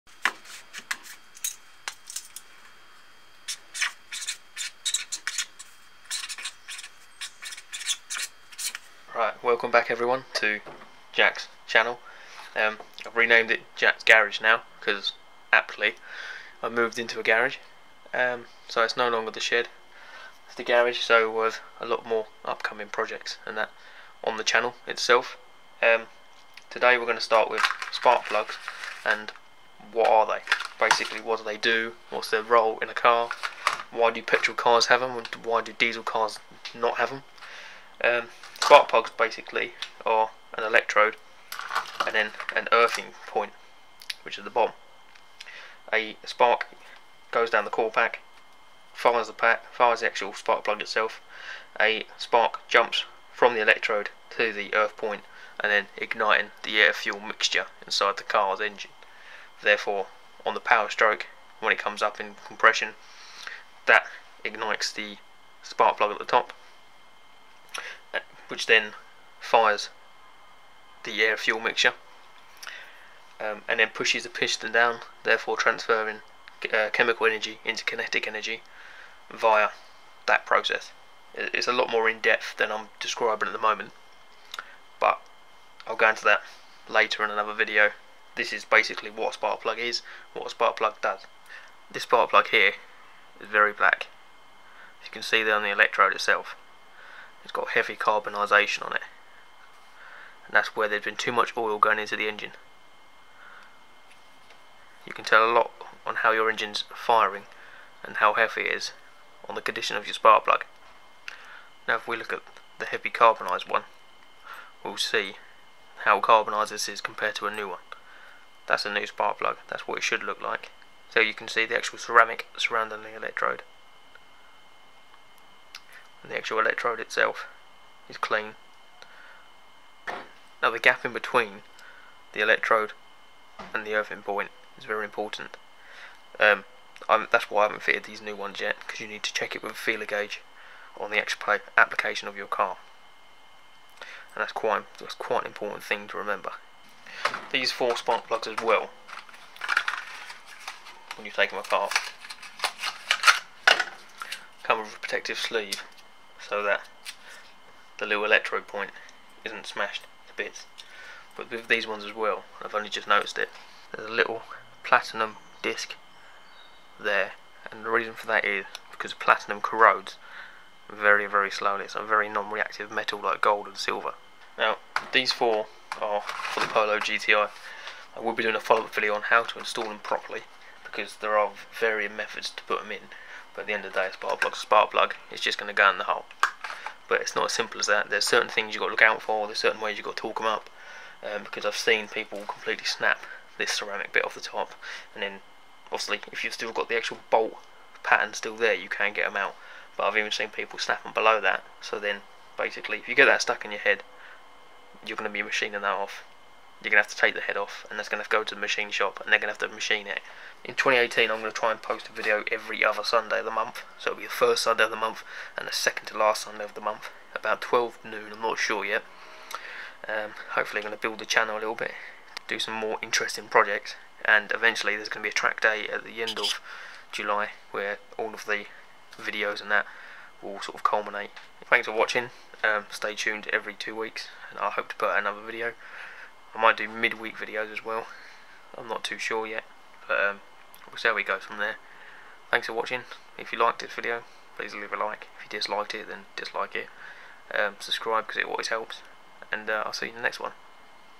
what is a spark plug? + channel update 2018
In the first video of 2018 I explain briefly what a spark plug is.
I am going to do a follow up video on how to install the spark plugs properly
Equipment used:
Go pro Suction cup mount
Go pro session
Nikon D5300 body
Nikkor 18-55mm 1:3.5-5.6GII lens
Rode VideoMicro Compact On Camera Microphone